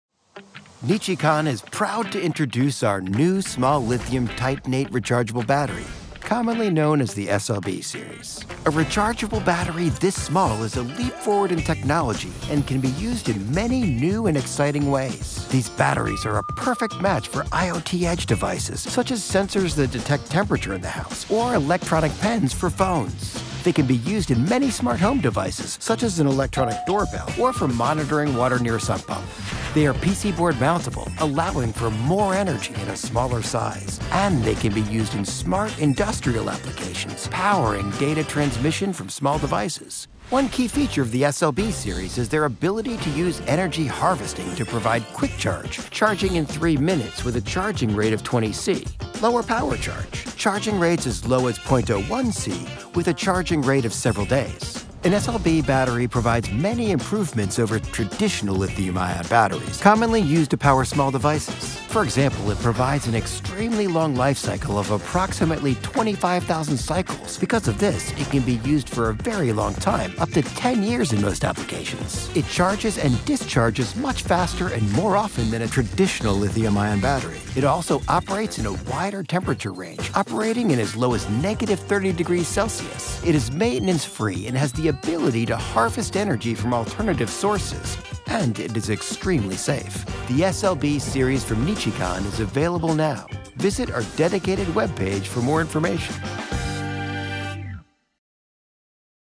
Nichicon NEW Lithium Titanate or LTO Battery
The NEW Lithium Titanate (LTO) Battery has many new adaptations and capabilities. This video helps showcase some of them.